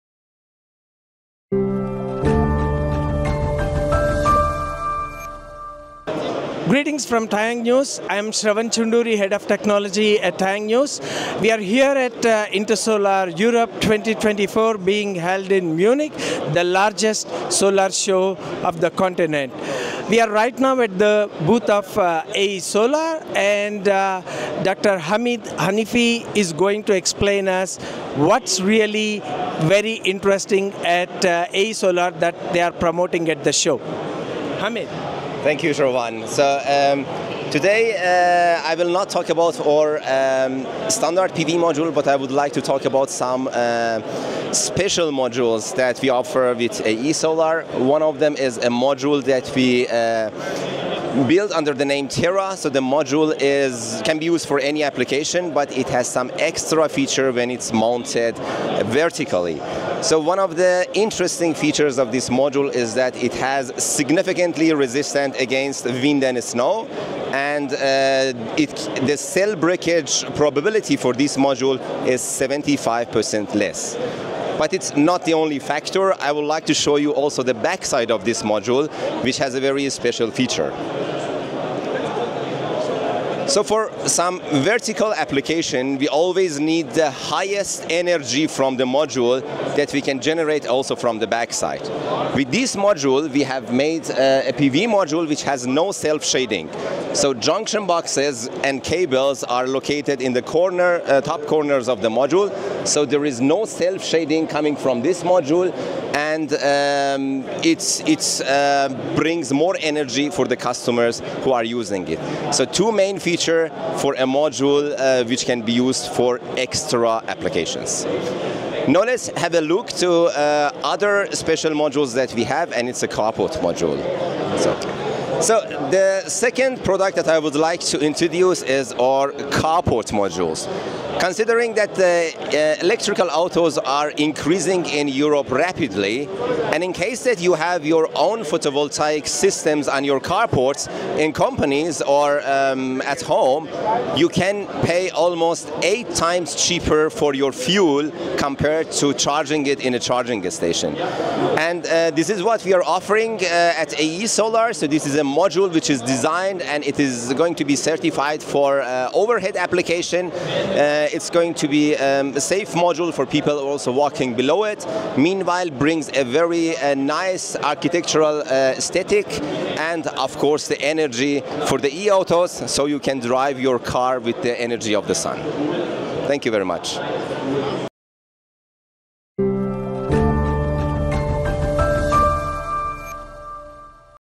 AESOLAR Speaks About Its Solar PV Products To TaiyangNews
At the Intersolar 2024 Exhibition in Munich, the TaiyangNews team interacted with several solar PV companies to learn about their latest product offerings. The smarter E Europe 2024, under which the Intersolar Europe Trade Fair was held, ended in Munich, Germany on June 21, 2024. The event featured 3,008 exhibitors from 55 nations, who displayed their products and services across 19 exhibition halls.

This video provides a glimpse of some of the solar PV products exhibited at the event. Over 110,000 industry professionals attended the show from 176 countries.